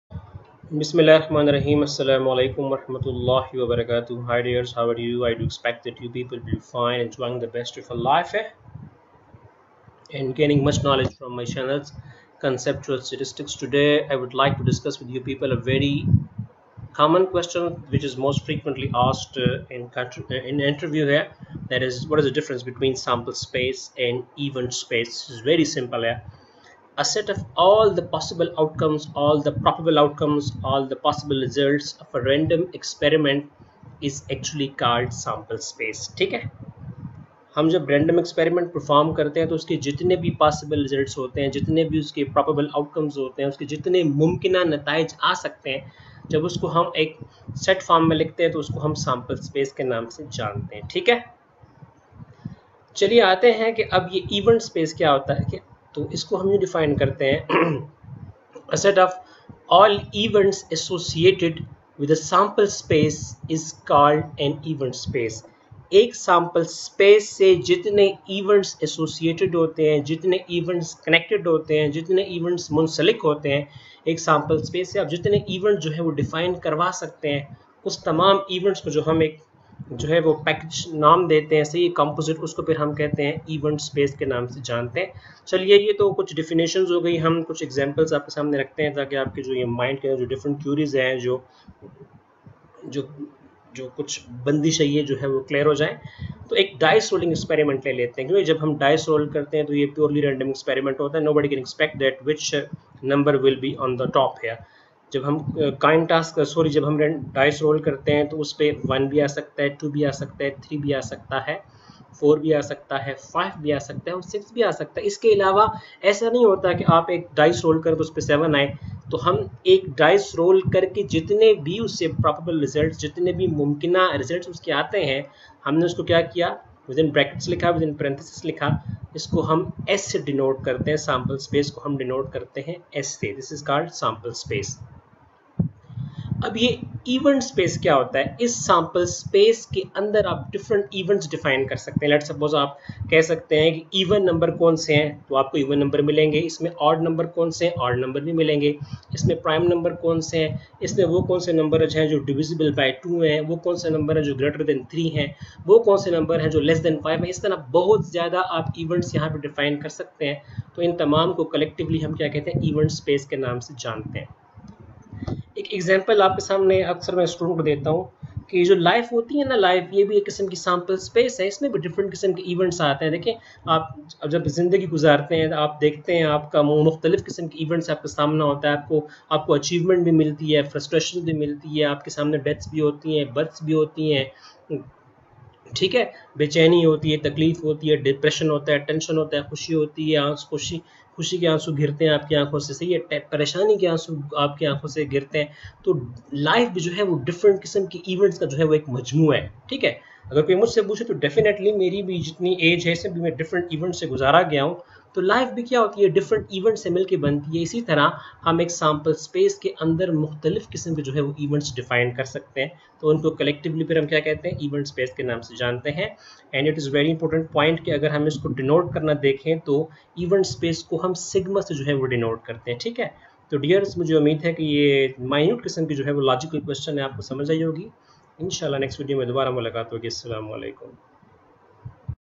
Sample space vs Event space😏 | Practical Examples✍
in this smallest video i explained the very common difference between SAMPLE SPACE AND EVENT SPACE. These are probability associated concepts. learn and stay blessed